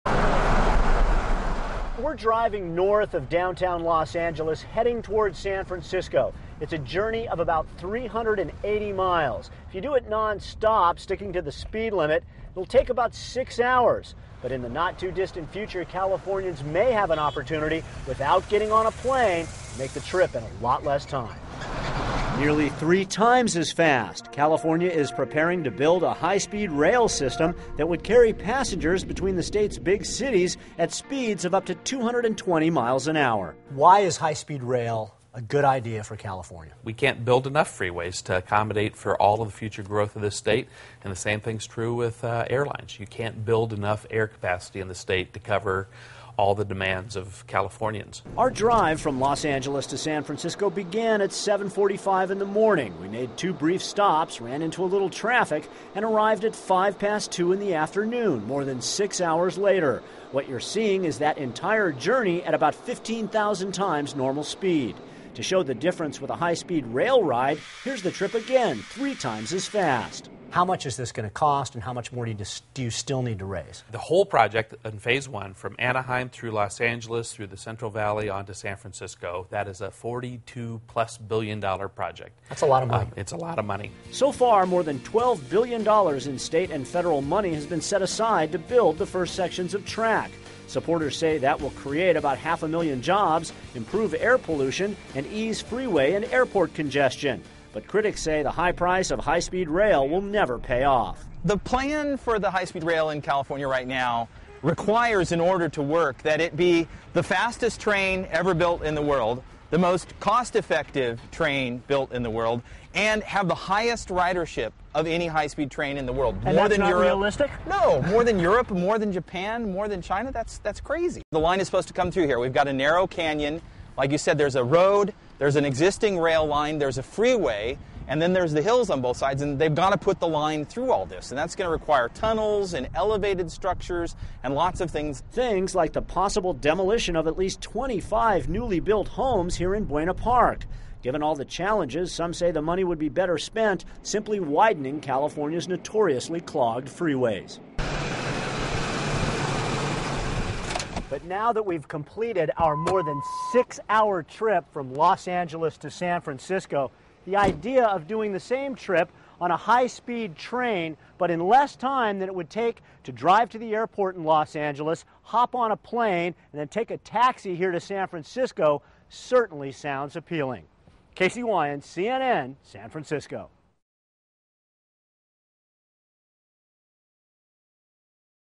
High-speed train skips traffic jams
California high-speed rail may ease travel woes but its steep cost worries critics.